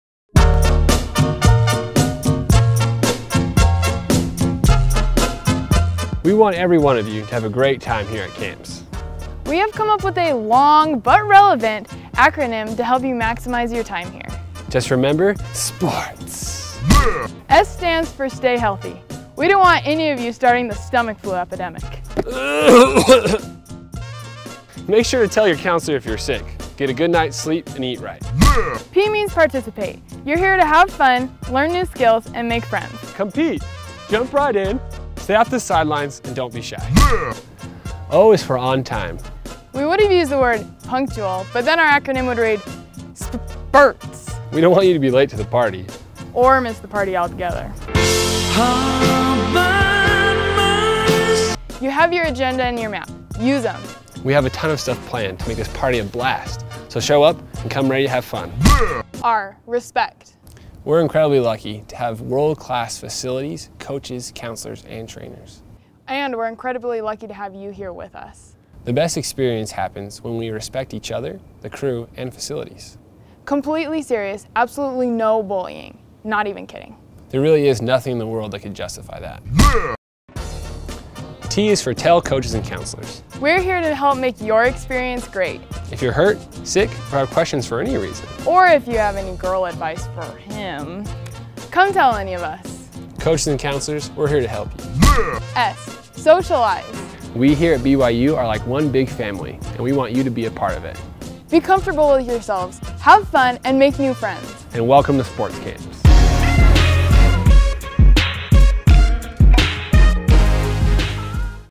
BYU Sports Camps - Enjoying Camps
Wanna make the most out of your camp experience? Here are some helpful tips from our crew to make sure you have a good time.